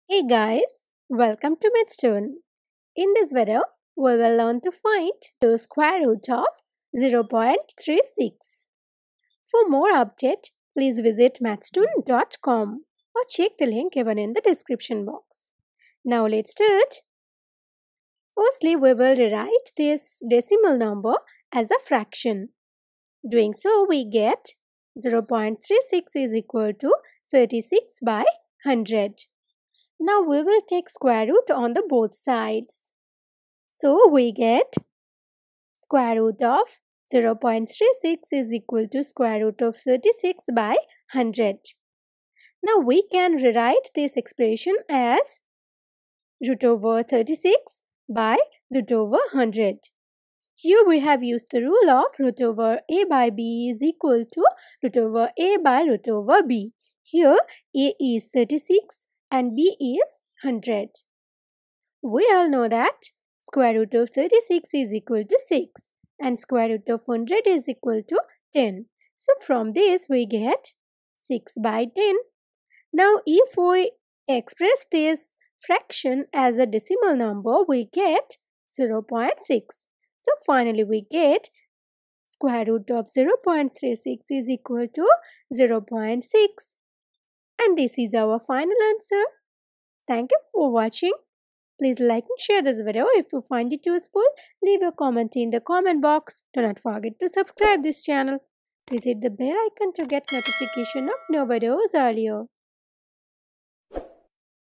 Square root of 0.36
In this video, we will find the square root of .36 in a very easy way.

We post videos daily on youtube